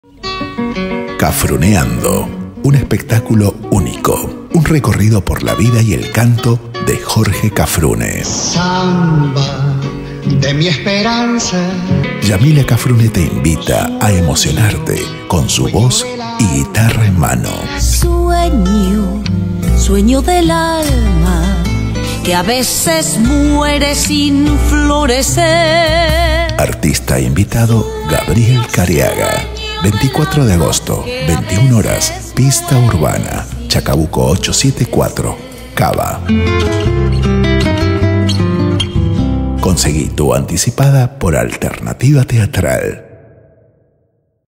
YAMILA CAFRUNE presenta CAFRUNEANDO
24 de Agosto, 21hs *@pista_urbana* Chacabuco 874 CABA

“La sangre no es agua” dicen por ahí y Yamila Cafrune vuelve a poner en escena un espectáculo muy querido por ella y por el público que lo disfrutó en distintos escenarios del país: CAFRUNEANDO, un recorrido por la vida y canto de Jorge Cafrune, su padre. En guitarra y voz, Yamila recorre un repertorio clásico de nuestro folklore argentino para emocionarnos y compartir historias de nuestra cultura popular.